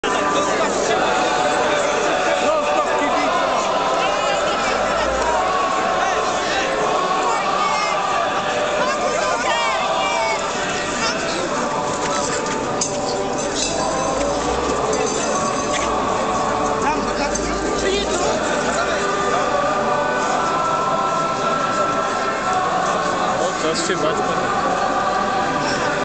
KRZYŻ-AWANTURA
Krzyżacy wynieśli mężczyznę skandującego: "precz z PIS!"